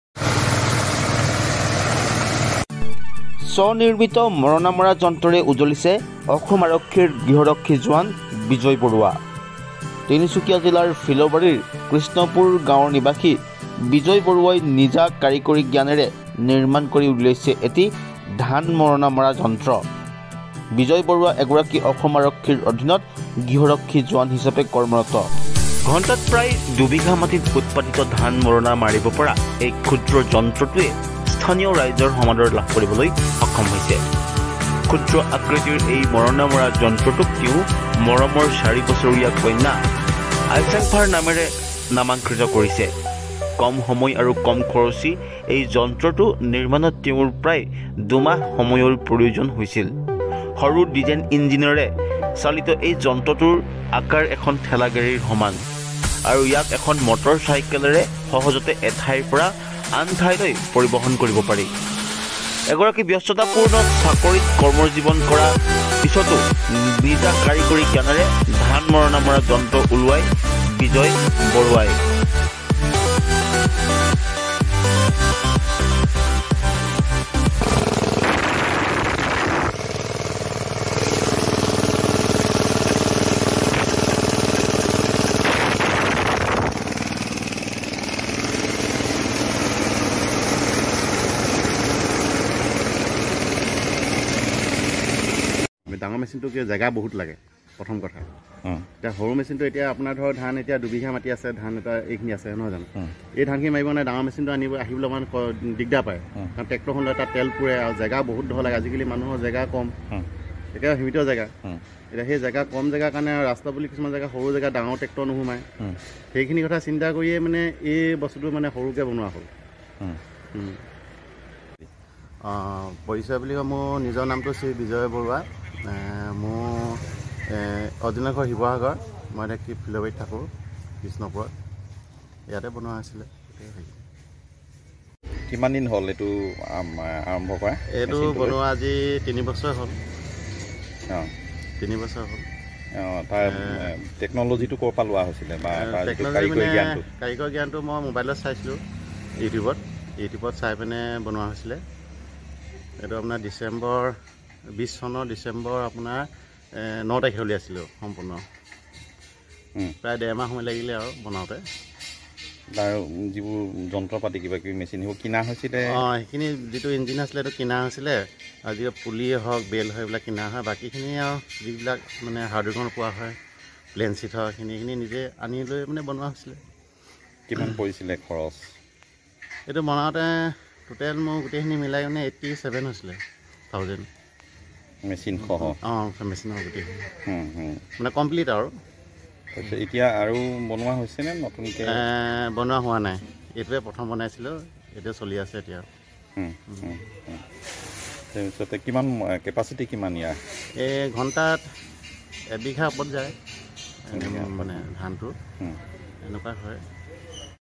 Dibrugarh News : এগৰাকী আৰক্ষীৰ অদ্ভূত আৱিস্কাৰ, নির্মাণ কৰি উলিয়াইছে এটি বিশেষ যন্ত্ৰ।
Dibrugarh News | এগৰাকী আৰক্ষীৰ অদ্ভূত আৱিস্কাৰ, নির্মাণ কৰি উলিয়াইছে এটি বিশেষ যন্ত্ৰ। তিনিচুকীয়া জিলাৰ ফিল'বাৰীৰ কৃষ্ণপুৰ গাঁও নিবাসী বিজয় বৰুৱাই নির্মাণ কৰি উলিয়াইছে এটি মৰনা মৰা যন্ত্ৰ। বিজয় বৰুৱা এগৰাকী অসম আৰক্ষীৰ অধীনত গৃহ ৰক্ষী জোৱান হিচাপে কৰ্মৰত।